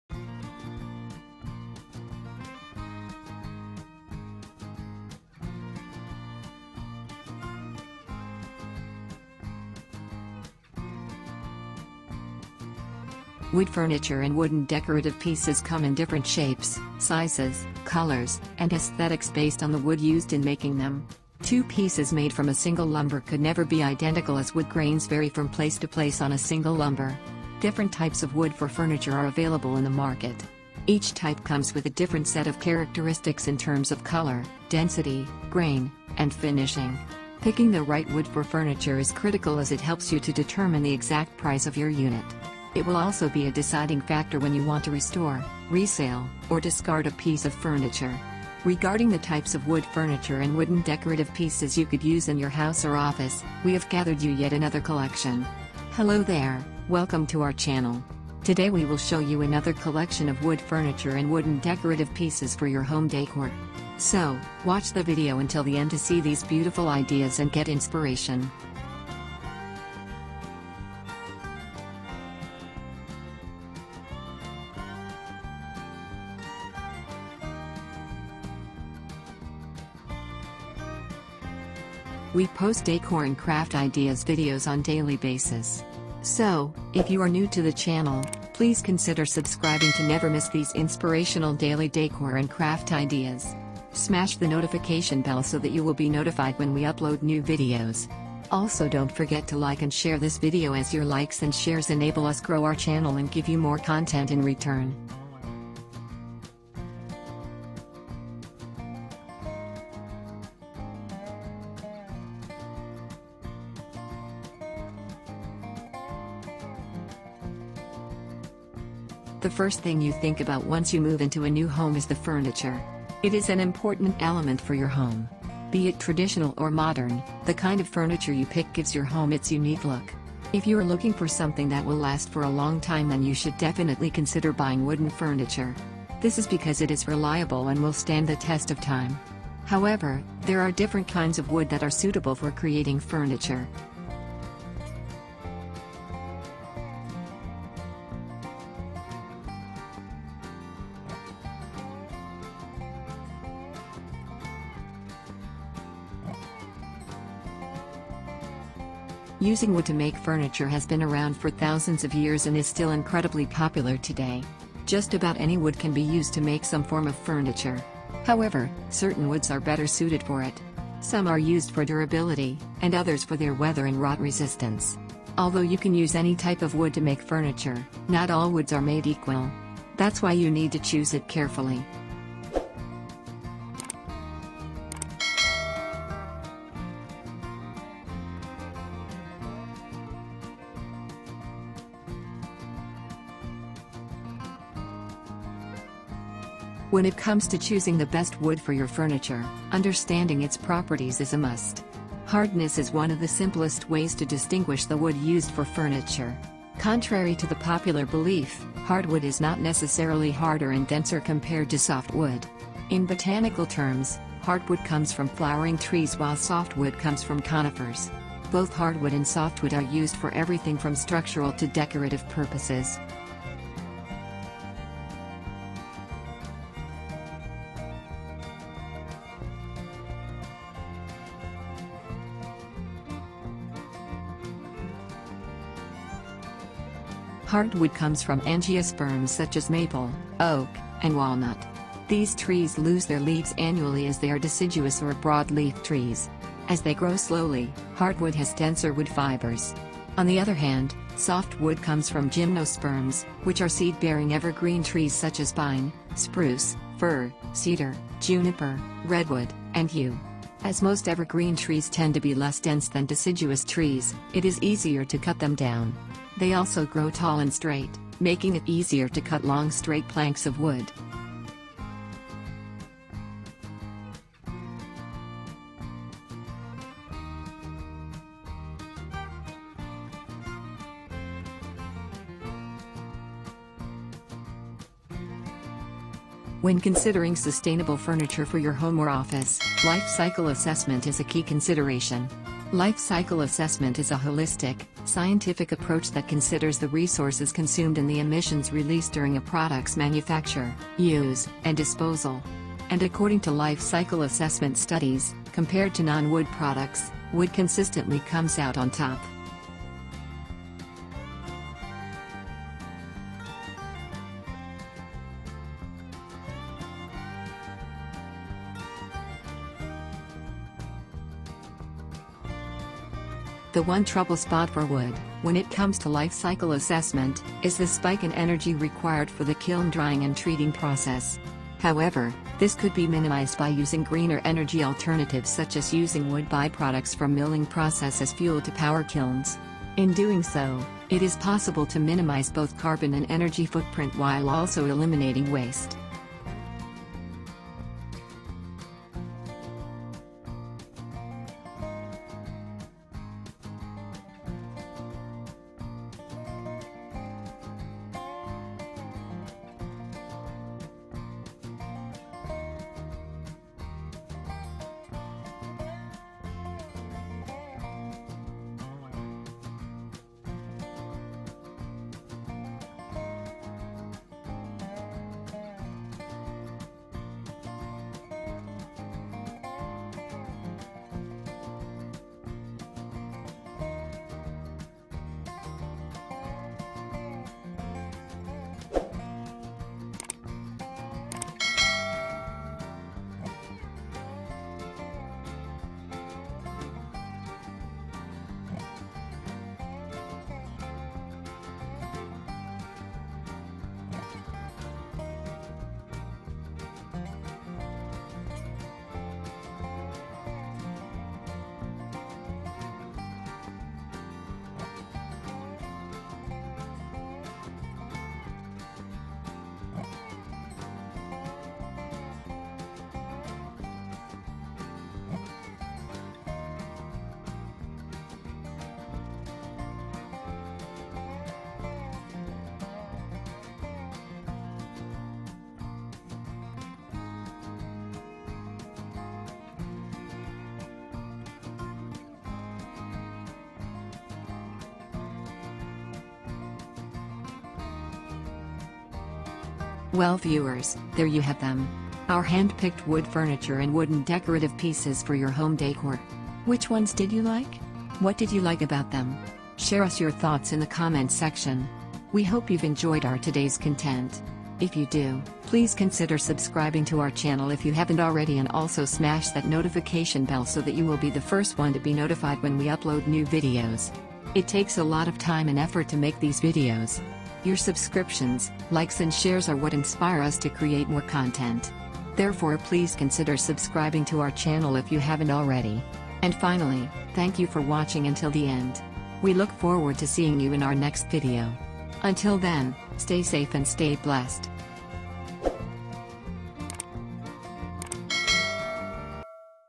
Wood furniture ideas and wooden decorative pieces ideas for home decor / Woodworking project ideas
Today we will show you a collection of Wood furniture ideas and wooden decorative pieces ideas for home decor/ Woodworking project ideas / Stylish woodworking ideas / make money woodworking ideas. Watch these useful wood furniture ideas and some wooden decorative pieces ideas. If you watch the video until the end you will see tons beautiful ideas and get inspiration.